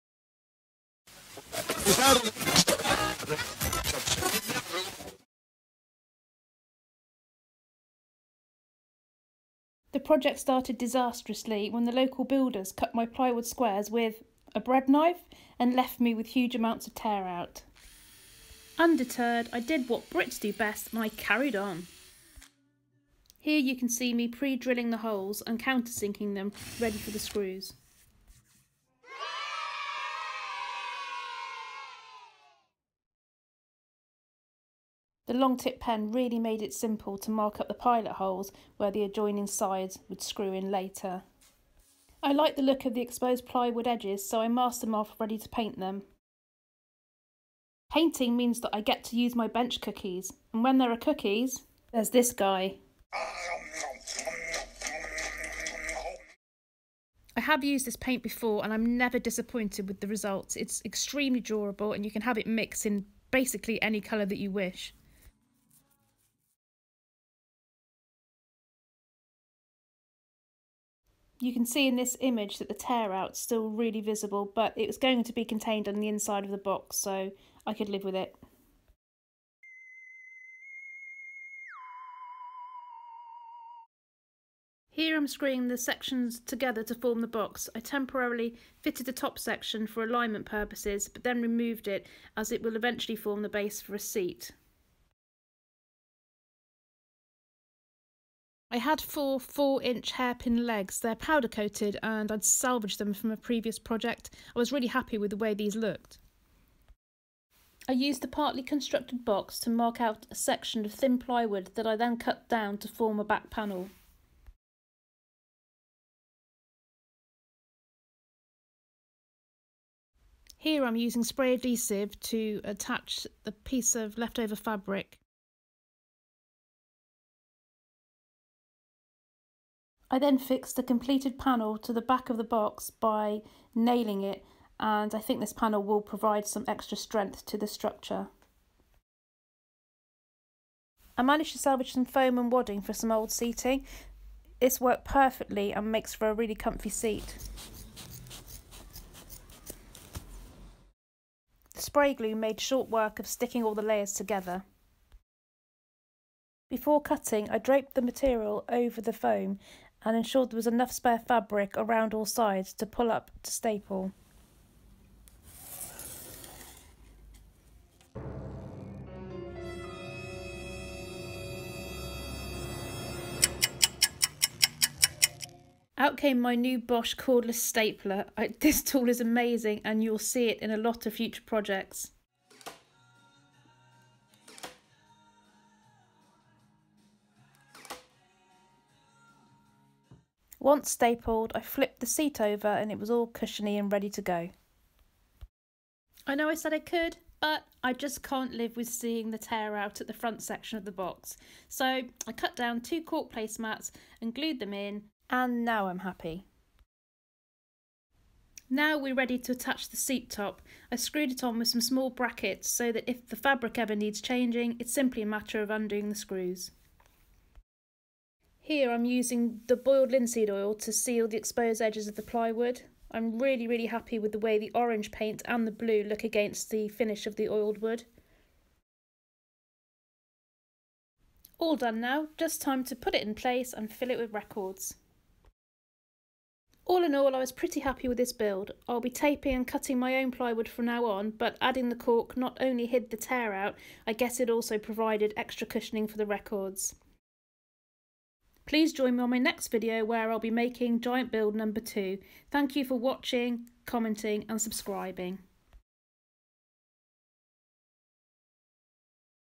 How to make a vinyl record storage seat with hairpin legs and plywood
I had a request to build a vinyl record storage for a messy teenager with a small room.

I decided to incorporate a seat - teenagers sit a lot, right?

Using salvaged plywood, salvaged foam and a small section of funky fabric I created this vinyl storage seat. I would like to make this project again and do better, but this was a great place to start, it used scrap materials and if it ever needs redesigning, the legs can be easily removed and reused.

If you like what you see, please give the project a thumbs up and click the subscribe button to make sure you keep up to date with all my latest creations.